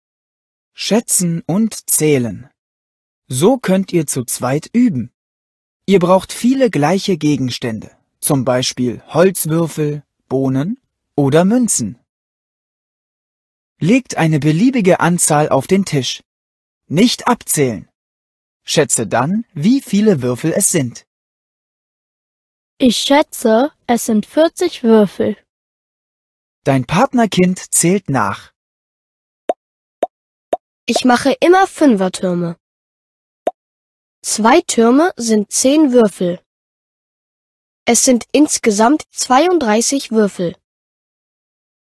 Schätzen“ einfach erklärt | Fredo BuchTaucher-App
Hier wird die Partnerübung „Schätzen und Zählen“ für Kinder der 2. Klasse verständlich erklärt. Die Übung trainiert das Schätzen von Anzahlen.

Bildquellen:
Evelyn Scherber

Weitere Urheber:
Tonstudio: audioberlin – The Audiotainment Company J. Ullmann & M. Scheuer GbR
Parametrisierung und Programmierung: KIDS interactive GmbH – Die Agentur für junge Medien, Erfurt